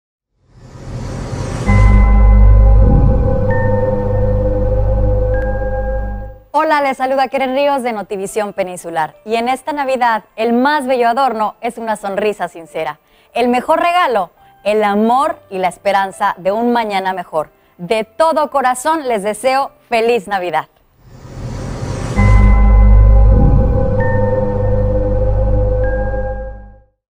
Keren Ríos te desea Feliz Navidad
En esta Navidad el más bello adorno es una sonrisa sincera el mejor regalo el Amor y la Esperanza de un mañana mejor.

De Todo Corazón Feliz Navidad.